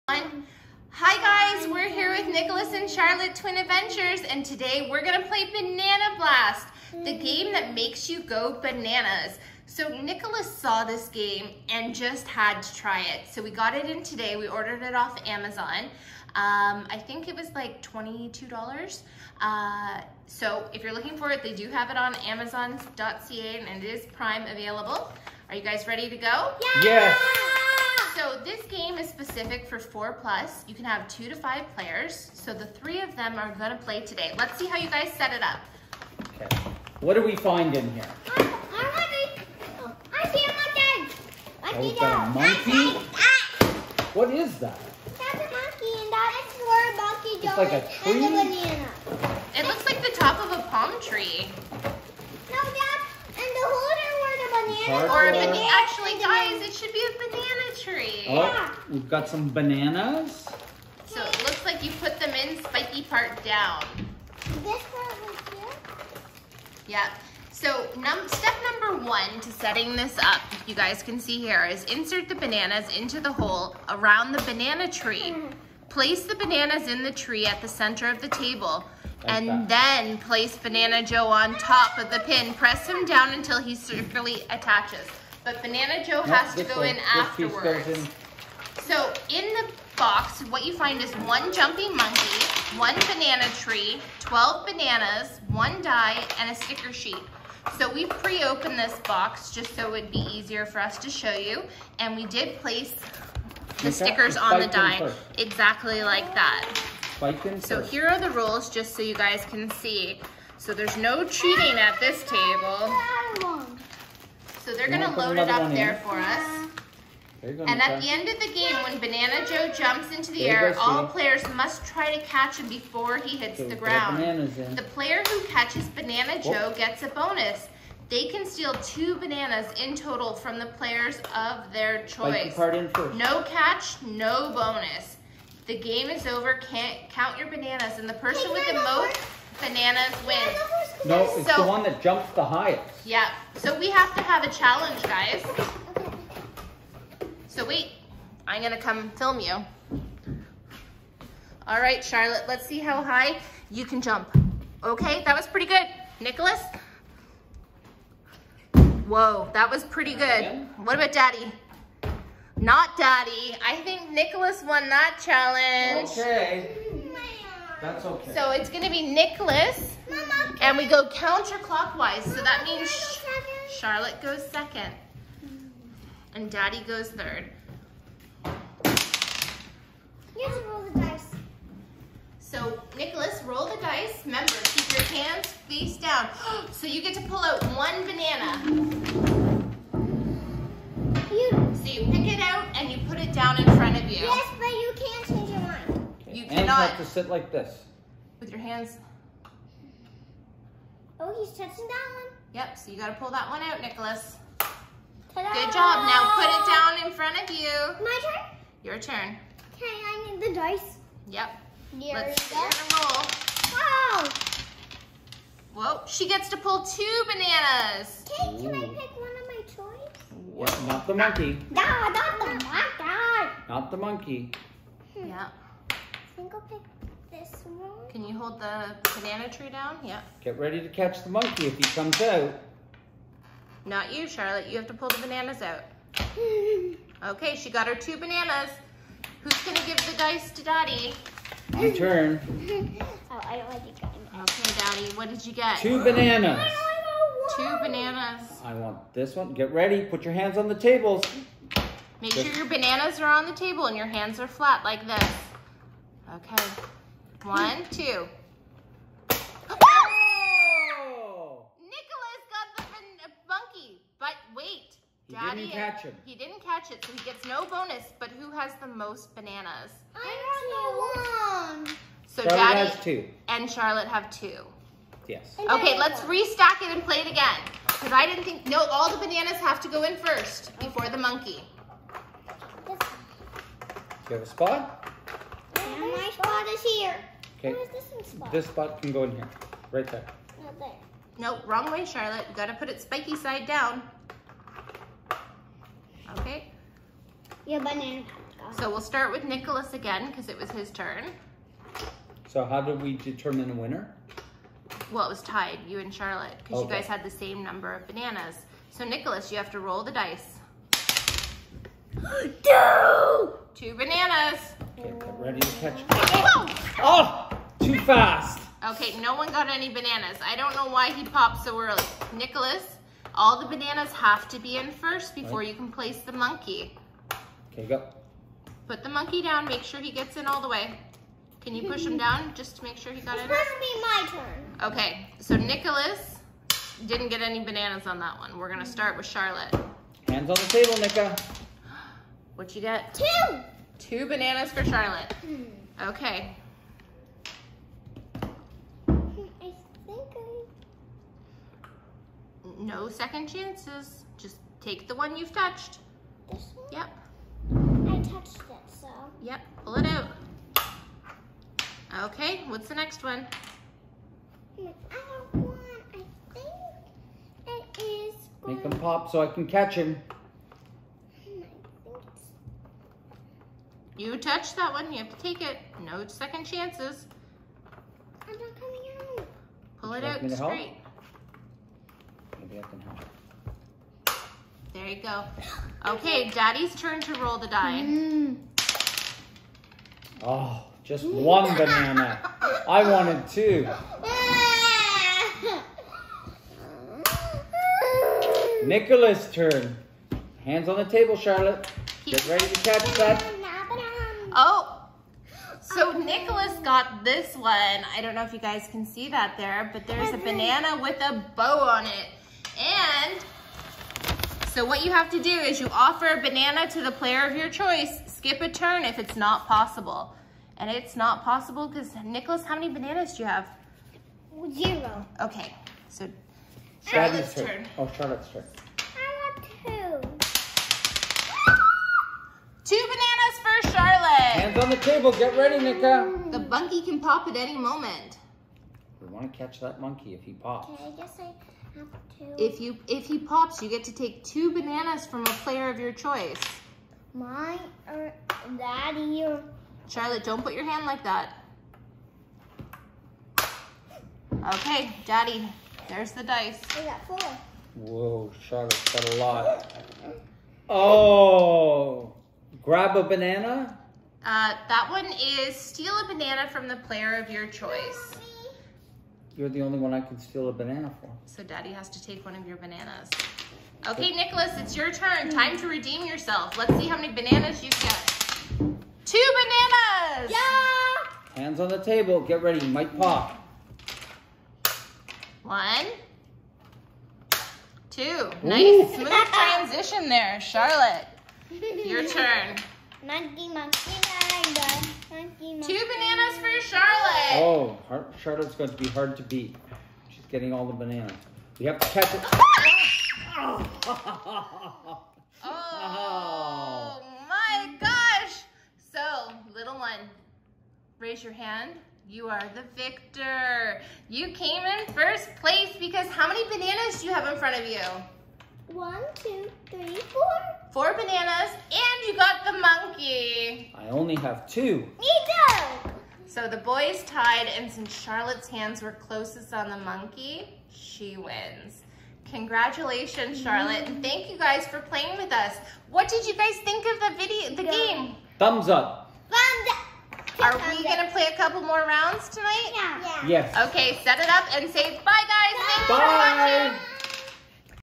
Banana Blast - Goliath played by Nicholas & Charlotte
Nicholas Charlotte and daddy play banana blast by Goliath